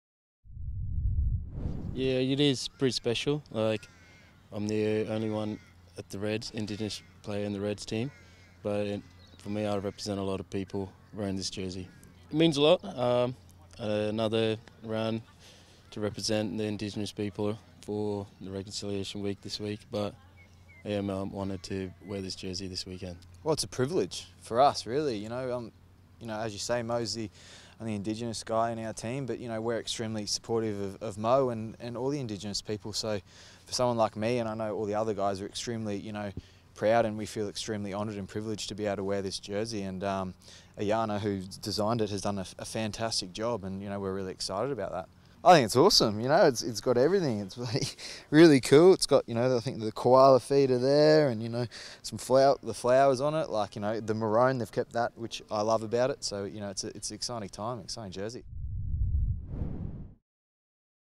Reds Unveil 2019 Indigenous Jersey
Proud QRU Indigenous Progam Ambassadors Moses Sorovi and Bryce Hegarty show off the 2019 St.George Queensland Reds Indigenous jersey ahead of this weekend's match agianst the Jaguares.